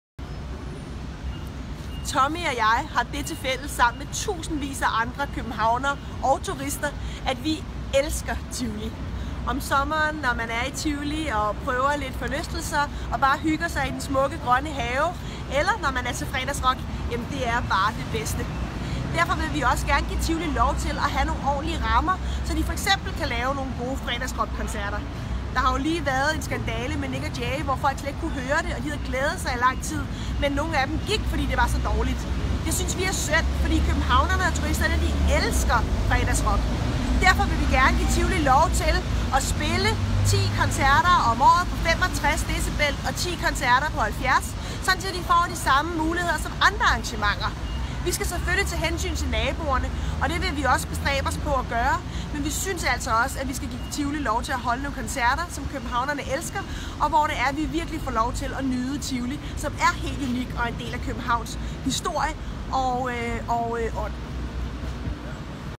Mærk Tivoli. Nyd musikken.
Vi elsker Tivoli. Det har Tommy og jeg til fælles med hundredvis af andre københavnere. Ikke mindst om sommeren, hvor vi hygger os i den smukke gamle have, prøver de sjove forlystelser eller nyder musikken til Fredagsrock.

Desværre er lyden til Fredagsrock så dårlig, at mange bliver meget skuffede, når de store spændende koncerter ellers går løs. Det er synd.

Fredagsrock er en unik mulighed for at høre store navne i smukke omgivelser.

Derfor ønsker vi, at Tivoli får samme muligheder for at spille musik, som byens andre arrangementer har.
Reglerne skal ændres, så Tivoli i 2018 kan spille gode koncerter, hvor vi kan høre musikken.

Vi skal selvfølgelig tage hensyn til naboerne. Men vi synes den rigtige balance er at give Tivoli lov til at skrue lidt op til glæde for tusindvis af koncertglade københavnere og turister.